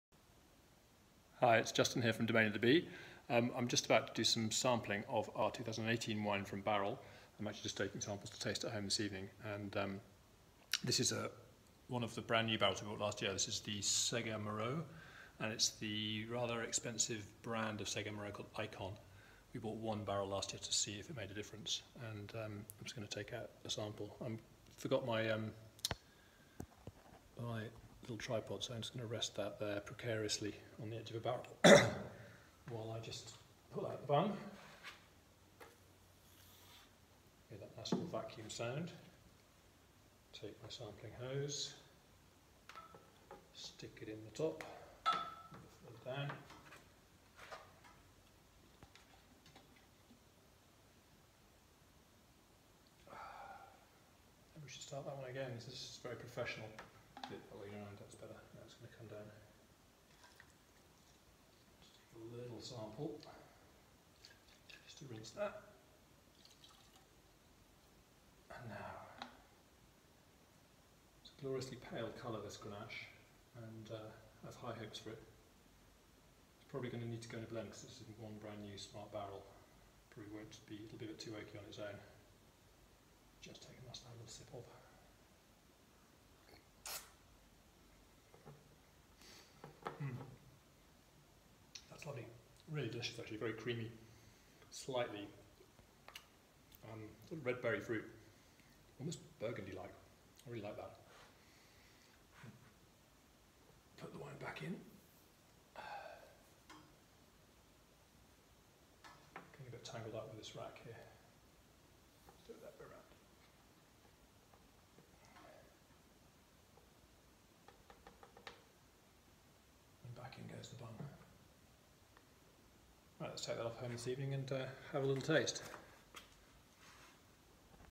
Taking samples (extremely professionally) from a barrel
Juggling the lack of a tripod and a dodgy siphoning technique, a sample of our best Grenache barrel.